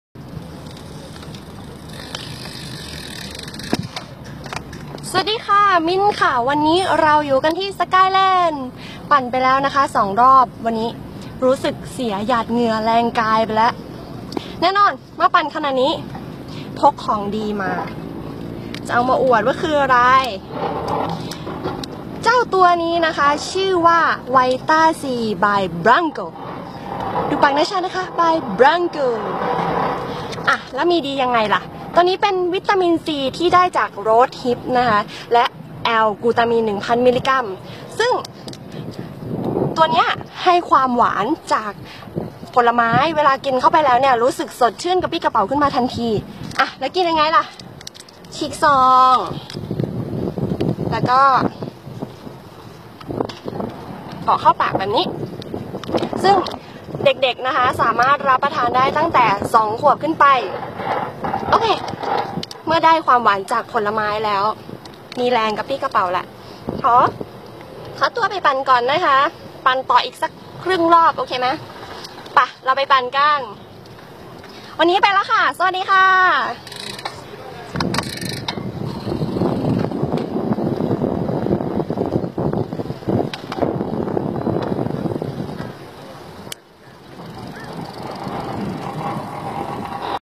น้องมิ้นต์นักปั่นสาวสวย รีวิววิตามินซี สกัดจาก Rose Hip
ฉีกทุกกฎเกณฑ์ของวิตามินซี ด้วย Opara Vita-C
วิตามินซีนำเข้าจากประเทศเกาหลี มีคุณค่าวิตามินซีสูง สกัดจากผลโรสฮิป